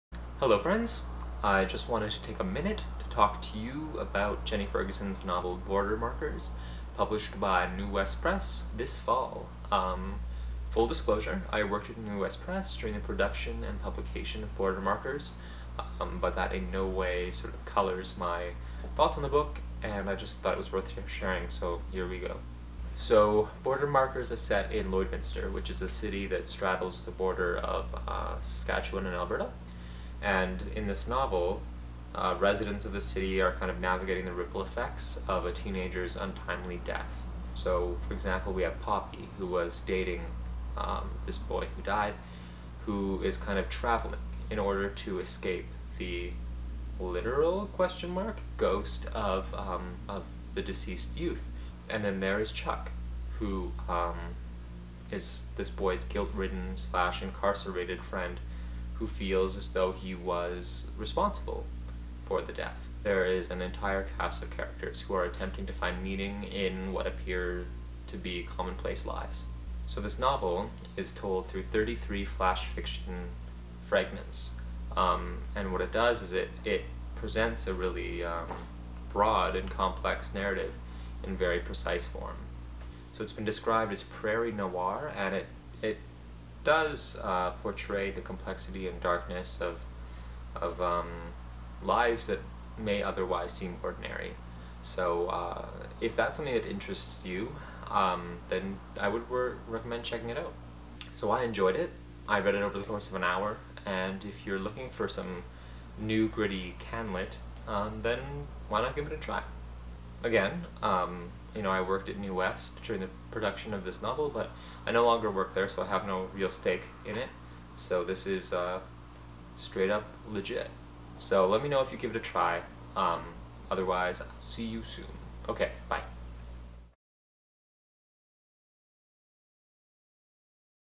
Talk | BORDER MARKERS by Jenny Ferguson
Talking about Jenny Ferguson's BORDER MARKERS published by NeWest Press.